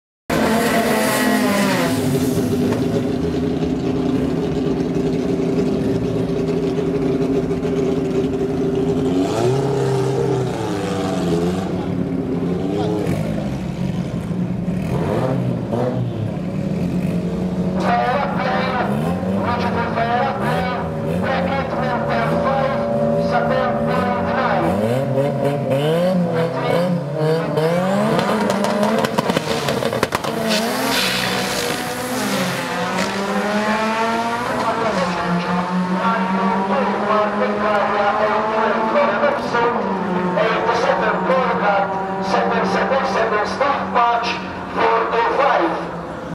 September 2017 QTM Day 3 Run 05
1st October 2017 - Day 3 (Eliminations and Finals) of the fourth Quarter Mile Event (The Horsepower Event) part of the 2017 MDRA championship.

After Eliminations where cancelled last week due to bad weather, this week we had a beautiful sunny day... so we where back in business! See you all in November For the Finals!